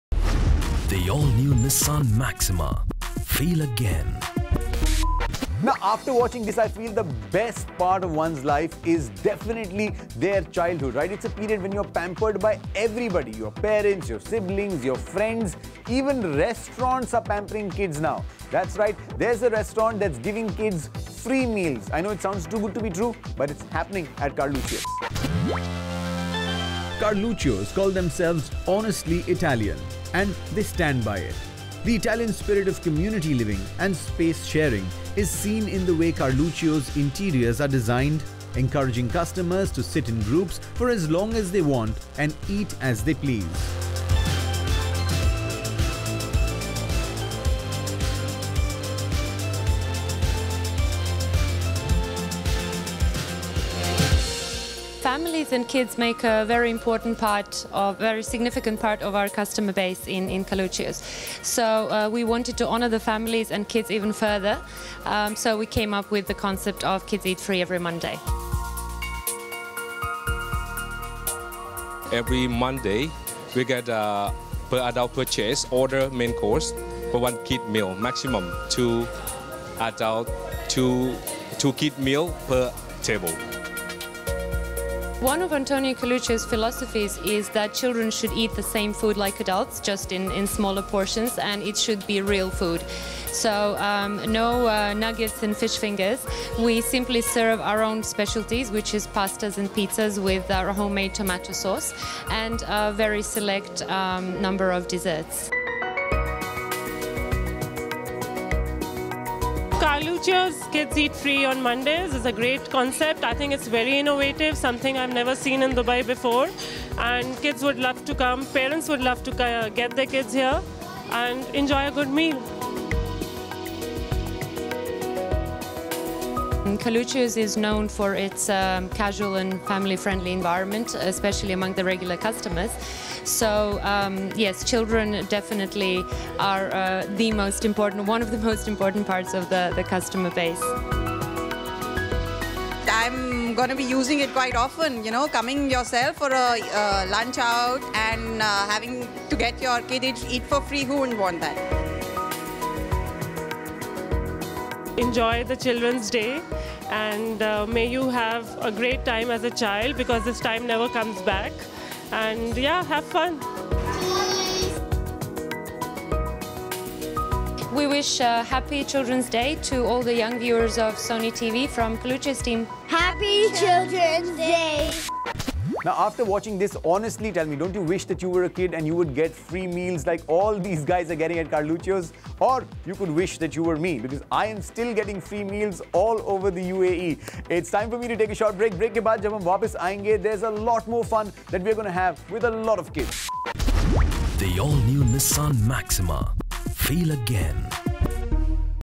Weekend Out with Gaurav Tandon - Carluccio's
Todays generation is surely amazing.. we could hardly decide our favourite chocolate at that age… but childhood in every generation is always the best times.. especially with the amount of pampering you get as a child, when we heard about the Monday free meal for Kids at Carluccios , we surely missed being a child… but here it is , so that your kids don’t miss out on it!